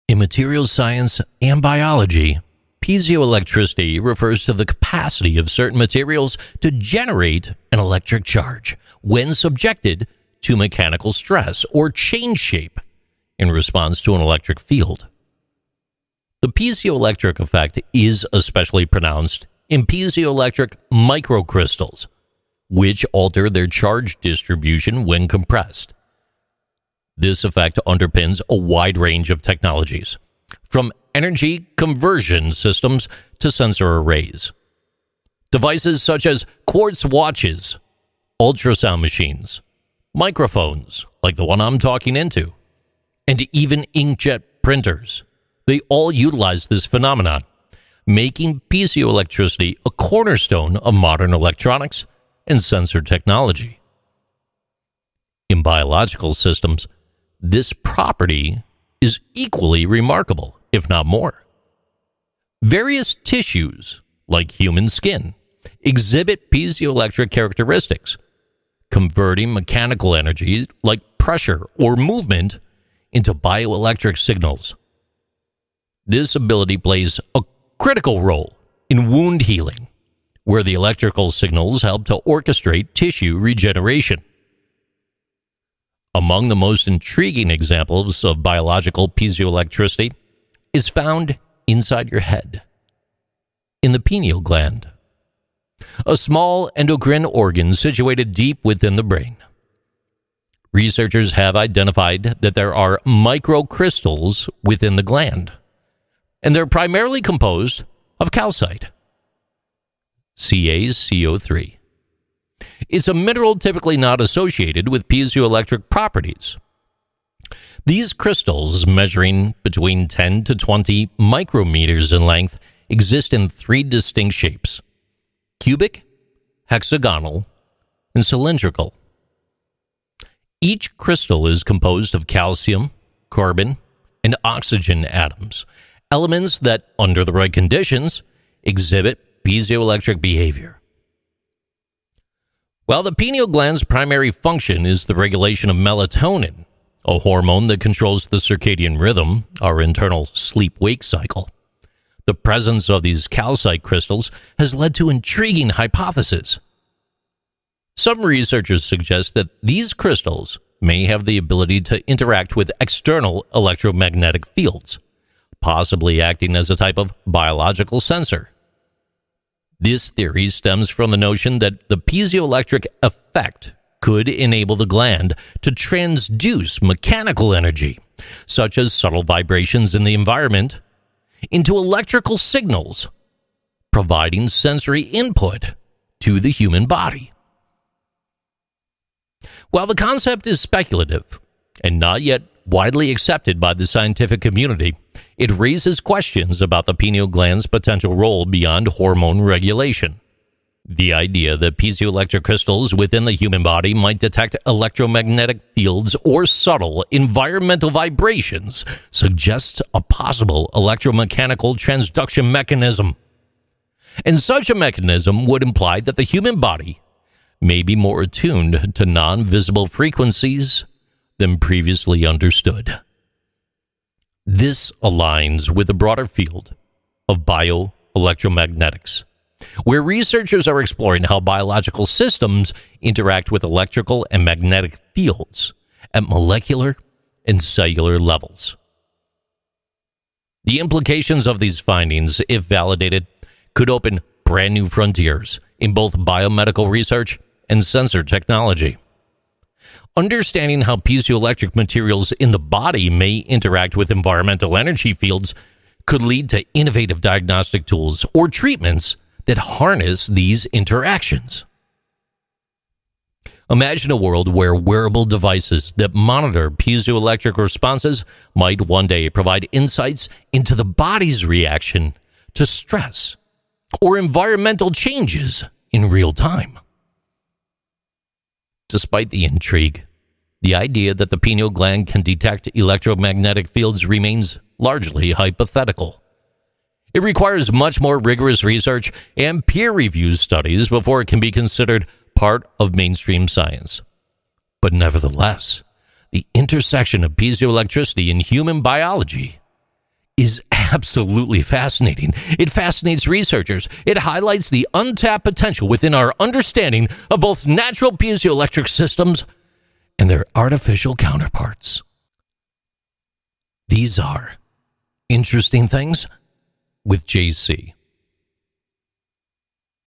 1072: "Piezoelectric Crystals & the Human Pineal Gland"
Interesting Things with JC #1072: "Piezoelectric Crystals & the Human Pineal Gland"

Discover the intriguing theory that piezoelectric crystals in the human pineal gland could interact with electromagnetic fields. Could our bodies be more attuned to unseen frequencies than we think?

Tune in @ThePodcastRadio or listen now at JimConnors.net